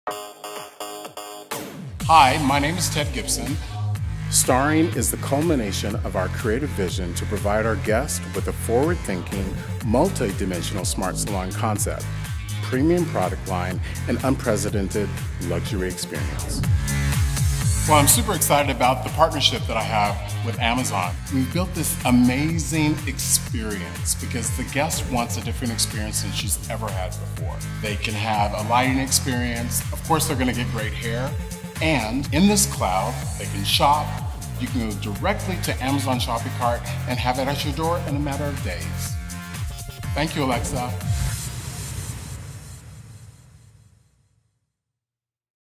STARRING By Ted Gibson
The Salon of the future the FIRST Smart Salon in the World powered by Amazon and Alexa

Celebrity stylist Ted Gibson and celebrity colorist Jason Backe
have opened the Los Angeles based
STARRING By Ted Gibson, the world’s first hair salon that merges
cutting edge smart home technology with the duo’s 30+ years of exceptional hair styling and color expertise.
Located at 211 ½ South La Brea, the spacious
1,100 square foot salon.

“STARRING is the culmination of our creative vision to provide consumers with a forward
-thinking, multi
-dimensional smart salon concept, premium product line, and unprecedented luxury experience all in one,”
shared Gibson. Adds Backe, “We’re excited to finally reinvent the largely outdated hair and salon industry,
and put for
 a new, disruptive way of thinking.”